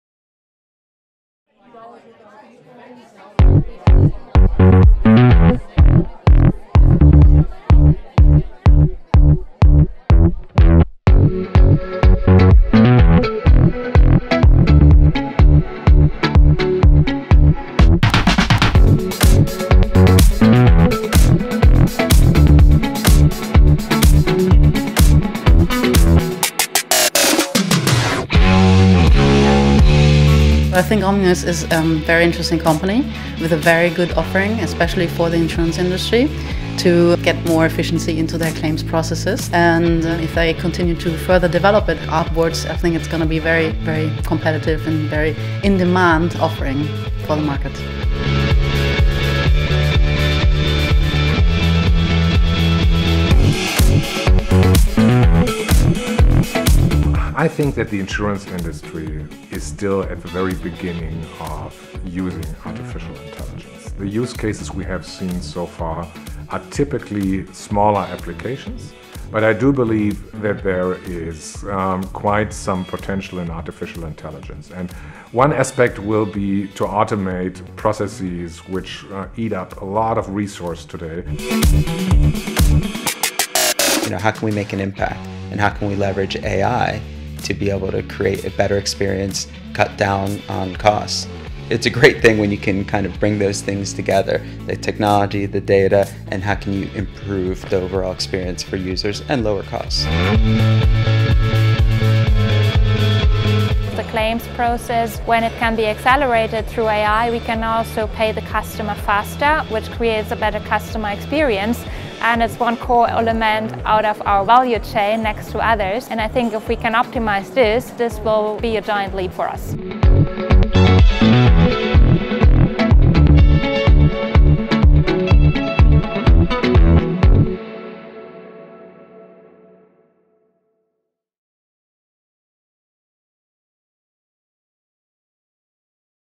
Machine Intelligence Summit 2018 | omni:us
An exciting day of discussion on all things AI and insurance, taking place at the heart of European technology, Berlin.